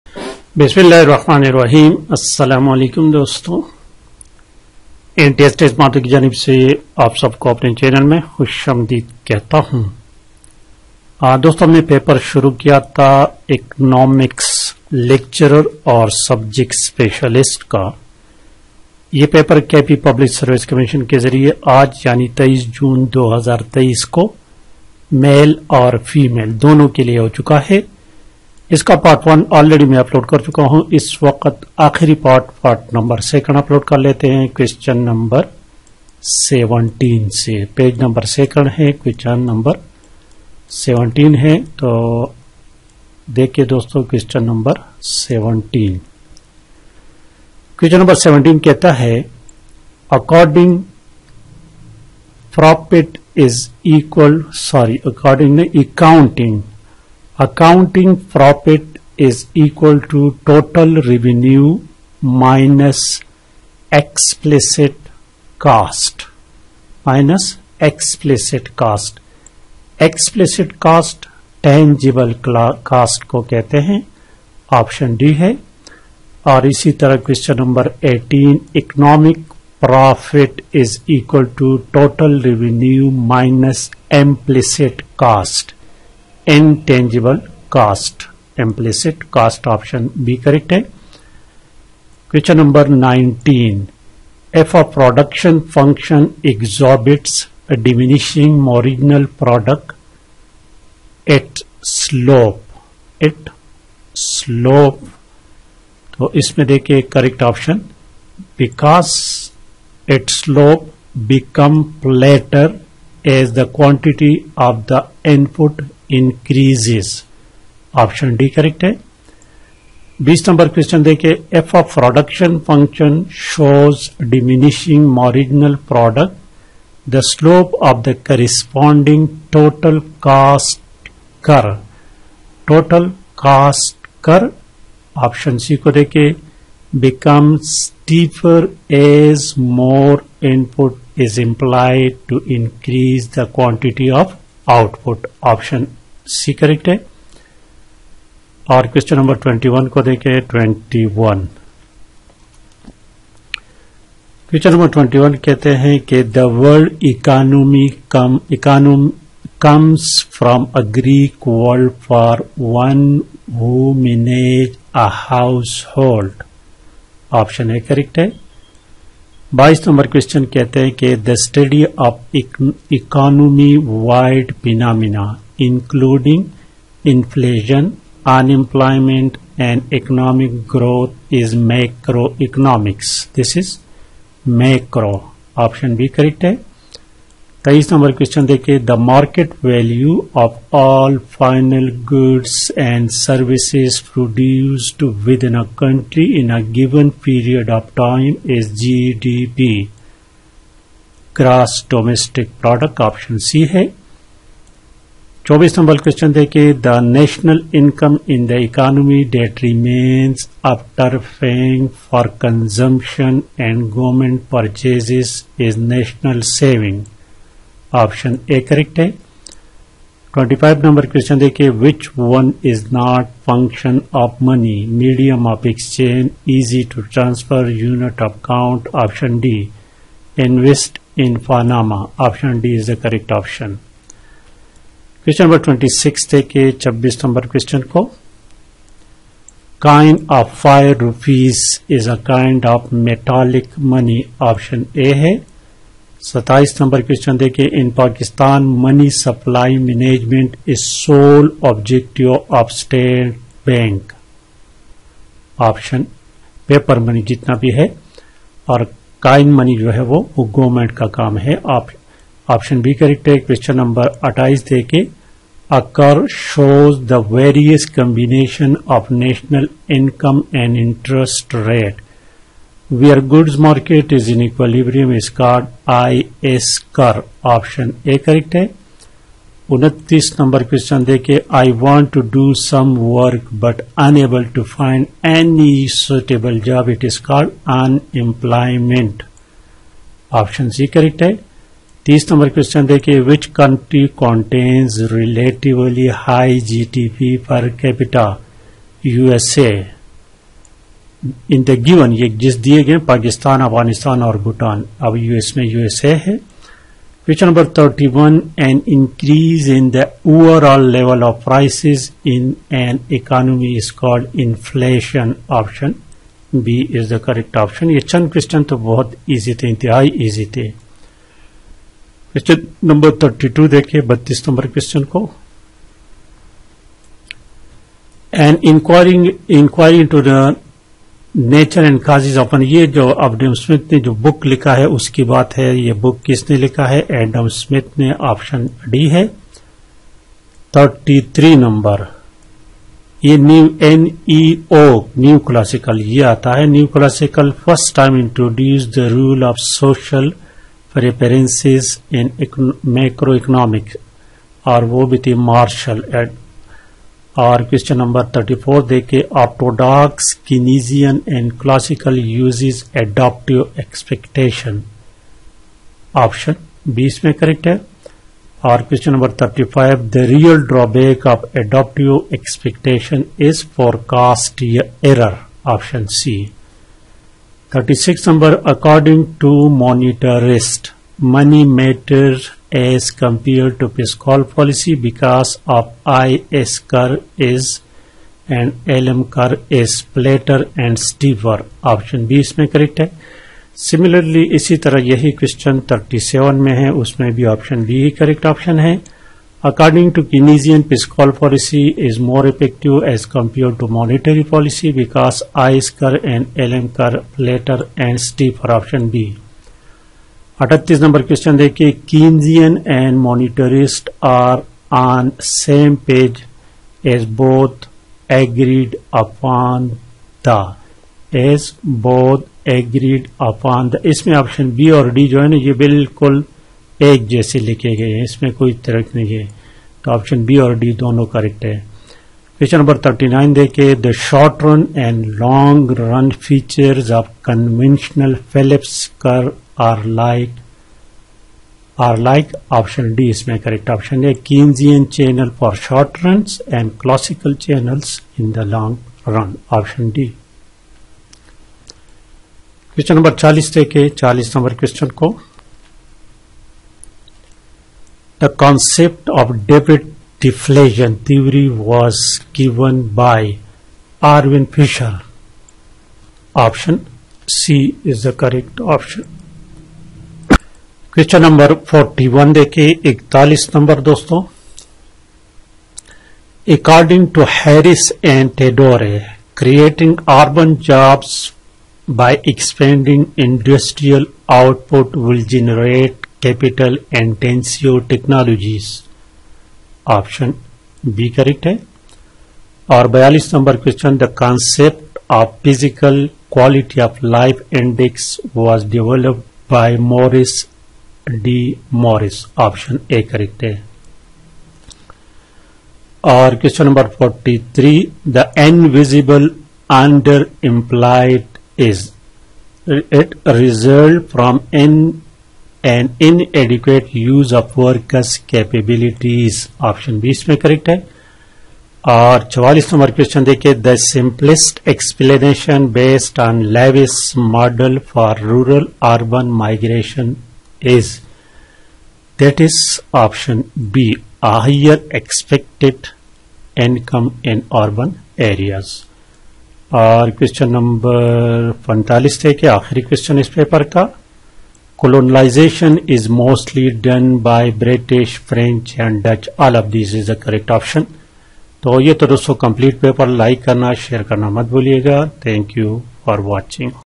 Lecturer / SS Economics KP PSC dated 23-06-2023 : Lecturer Subject Specialist in Economics Part - 2
A perfectly competitive market has.
If an increase in the price of good X leads to a decrease in the demand for good Y, then X and Y are
The law of demand states that an increase in the price of a good.
The las of supply states that an increase in the price of a good.
If an increase in consumer incomes leads to a decrease in the demand for good X, the good x is:
That the supply curve for good X is upward sloping indicate that.
Which of the following shifts the demand for good X to the right.
If the price of a good is above the equilibrium price.
If the price of a good is below of the equilibrium price.
If the price of a good is equal to the equilibrium price.
An increase (rightward shift) in the demand for a good will tend to cause.
A decrease (leftward shift) in the supply for a good will tend to cause
If there is an increase in both the supply and demand for computers. In the market for computer, we would expect.
Suppose there is an increase in both the supply and demand for personal computers. Further, suppose the supply of computers increases more than demand for computer. In the market for personal computer, we would expect.
Suppose consumer tastes shift toward the consumption of apples. What is the impact of this event on the market for apples.
Suppose both buyers and sellers of good x (for example wheat) expect the price of good x to rise in the near future. What would happen to the equilibrium price & quantity in the market for good x today.
accounting profit is equal to total revenue minus
Economic profit is equal to total revenue minus
If a production function exhibits diminishing marginal product, its slope
If a production function shows diminishing marginal product, the slope of the corresponding total cost curve.
The word economy comes from a greek word for
The study of economy-wide phenomena, including inflation, unemployment, and economic growth is.
The market value of all final goods and services produced with in a country in a given period of time is.
The total income in the economy that remains after paying for consumption and government purchases.
Which one is not a function of money.
Coin of Five rupees is a kind of money.
In Pakistan, money supply management is sole objective of:
A curve shows the various combination of national income and interest rate. Where goods market is in equilibrium is called.
I want to do some work but unable to find any suitable job. Is is called.
Which country contains relatively high GDP per capita
An increase in the overall level of prices in an economy is called.
An inquiry into the Nature and causes of the wealth of nations, generally referred to by its shortend title The wealth of nation was written by.
_________ first time introduced the role of social preferences in macroecomic
Orthodox Keynesians and classicals used _______ expectations.
The real drawback of adaptive expectation is:
According to monetarist, money matters as compared to fiscal policy because of IS curve is _______ and LM curve is ______
According to Keynesian, fiscal policy is more effective as compared to monetary policy because IS curve is _______ and LM curve is____
Keynesian and monetarist are on same page as both are agreed upon the:
The short run and long run features of conventional phillip’s curve are like.
The concept of debt deflation theory was given by
According to Harris and Todaro, creating urban jobs by expanding industrial output
The concept of Physical Quality of Life index was developed by
The invisible underemployed
The simplest explanation based on Lewis’s model for rural-urban migration is:
Colonization is mostly done by